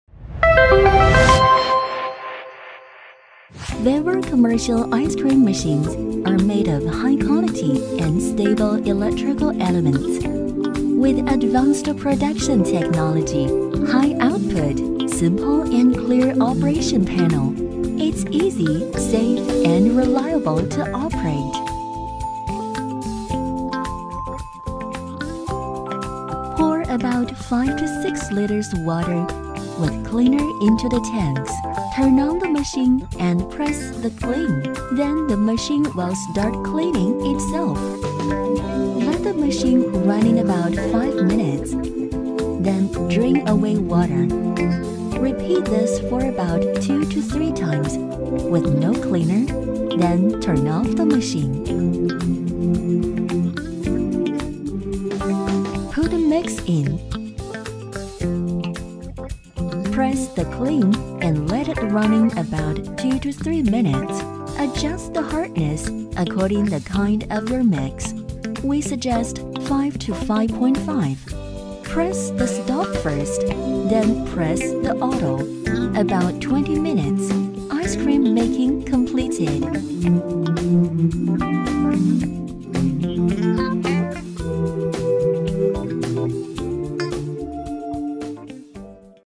How to make Ice Cream at home - Soft ice cream
Soft Ice Cream Machine Operation.
Convenient to operate with LED display ice cream machine. Multi-purpose usage for healthy & DIY desserts.
Signature service on delivery.
Ice cream machine with fast refrigeration, 4 powerful frozen evaporator, low temperature,high efficiency.